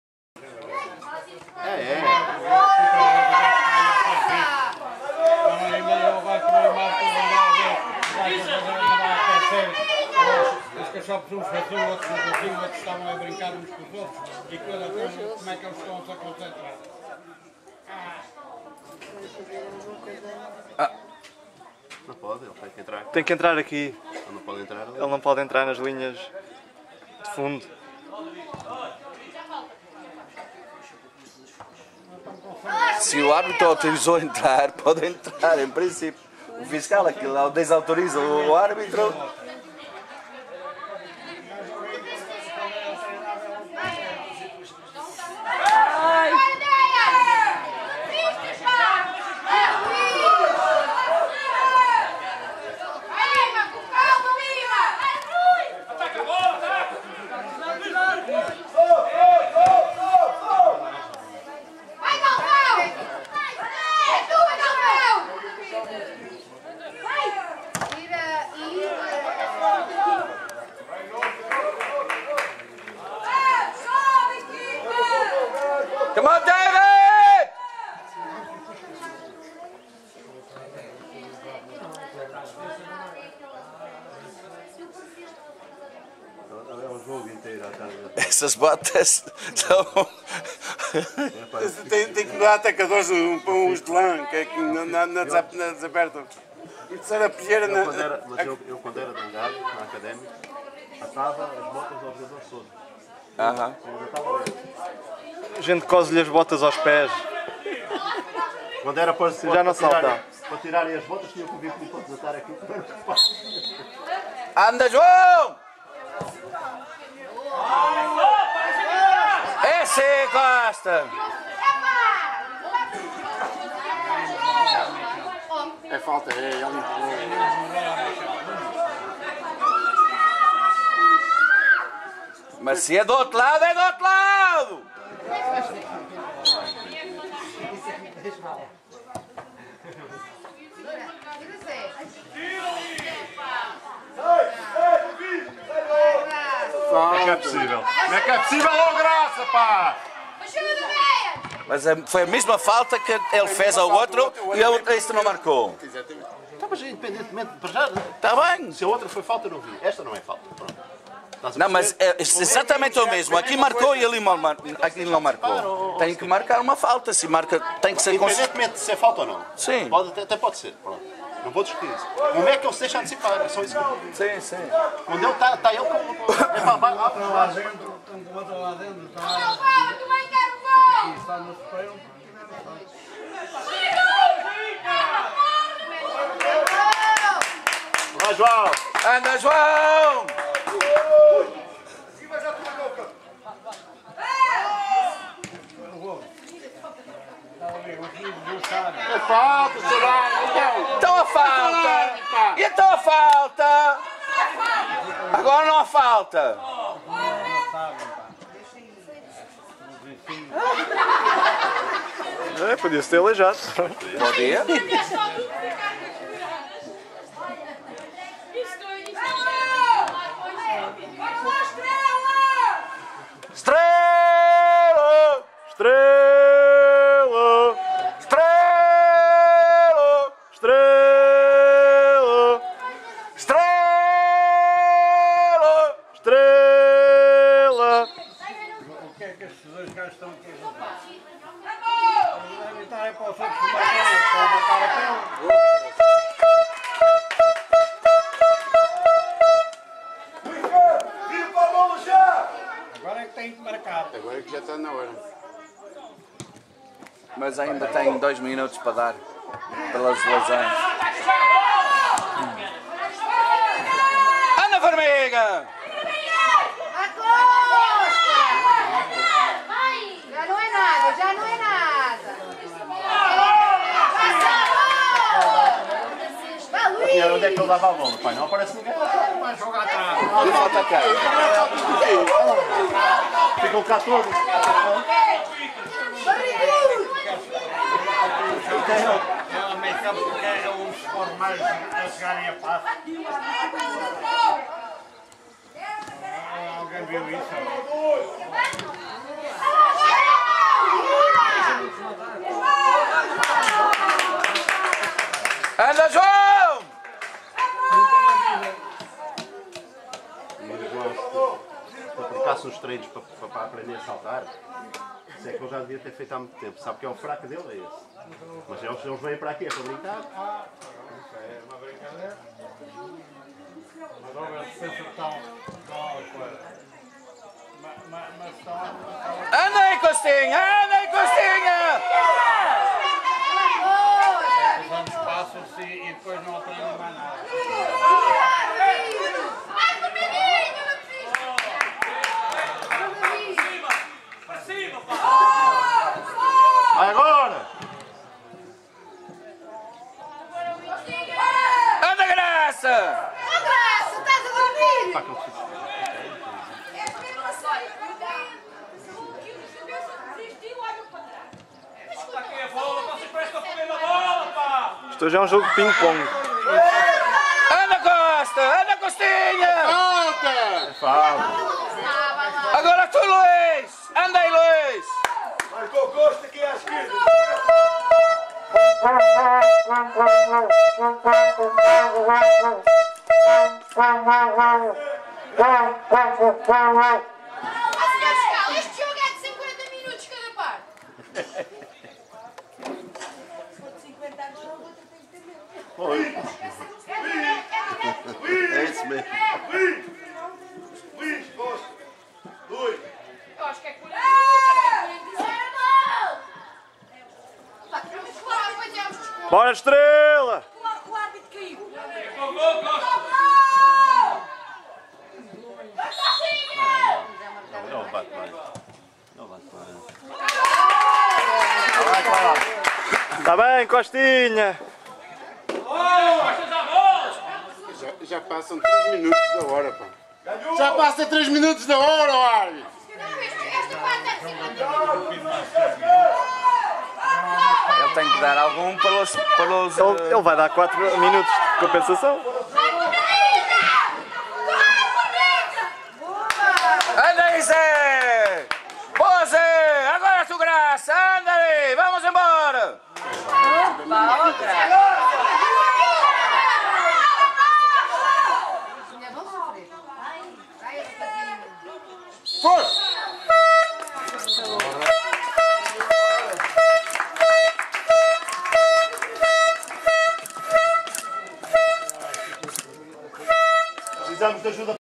Estrela FC Ouriquense 4-1 Fazendense (7 de 13)
13/5/2012: Estrela FC Ouriquense 4-1 Fazendense, Juvenis (época 2011/12)

Final da 1ª parte

Resultado ao intervalo: 2-1